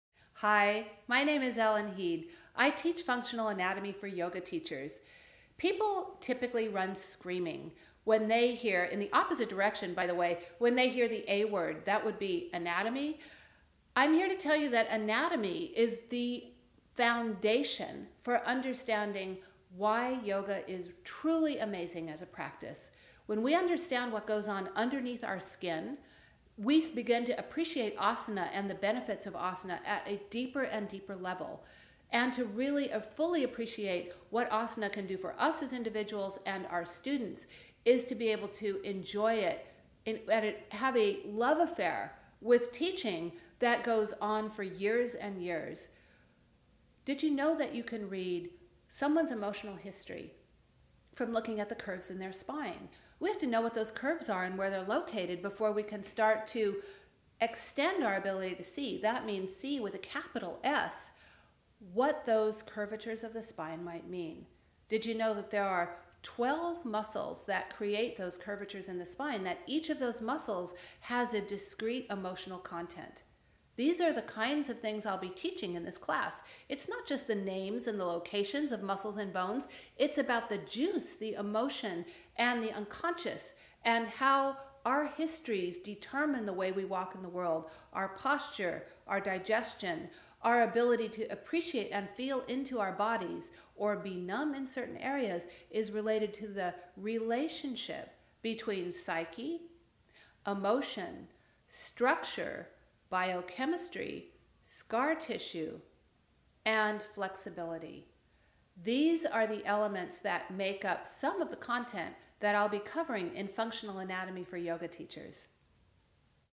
Functional Anatomy for Yoga Teachers
Ellen Heed takes you on a three day anatomy journey that will change the way you look at yoga.